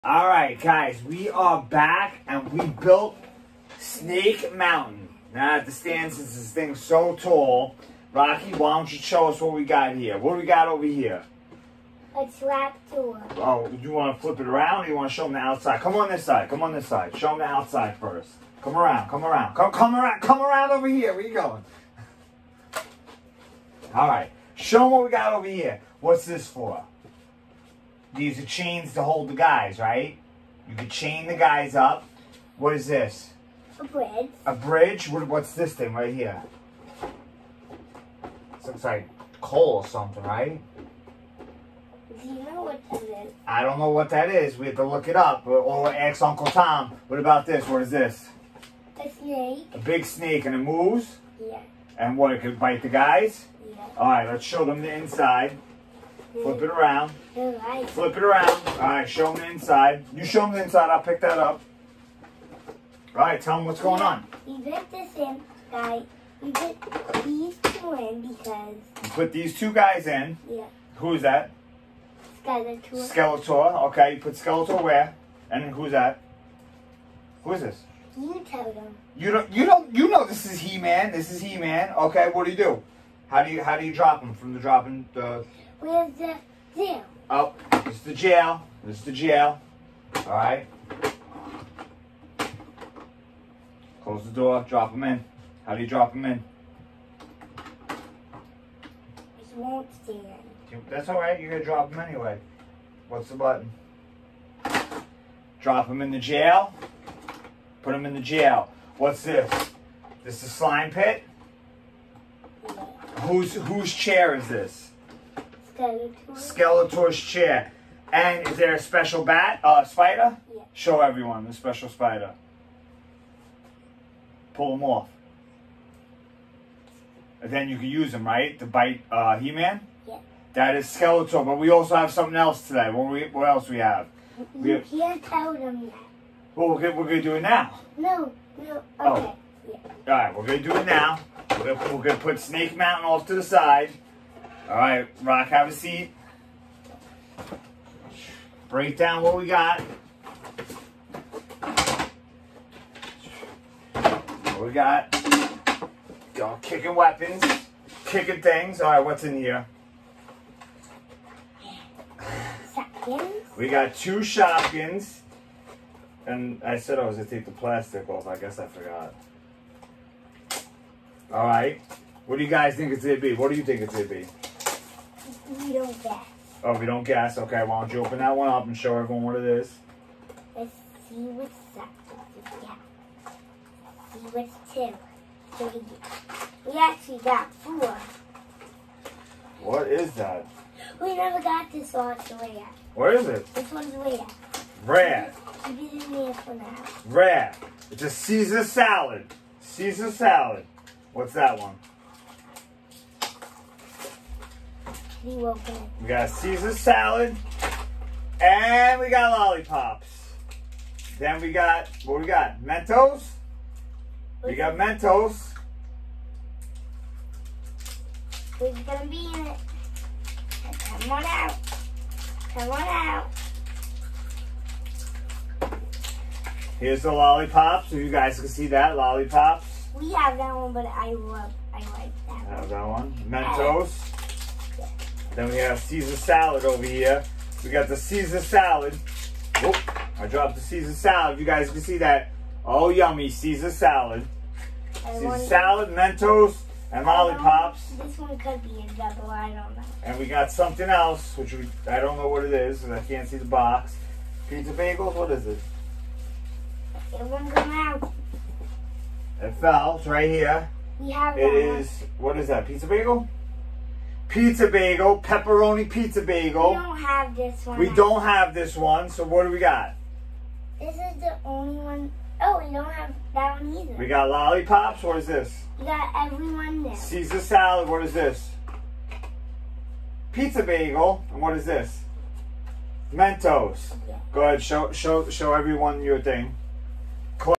Masters of the Universe’s Snake Mountain !!
Rocky opens up MOTU Snake Mountain , He-Man , toys , playset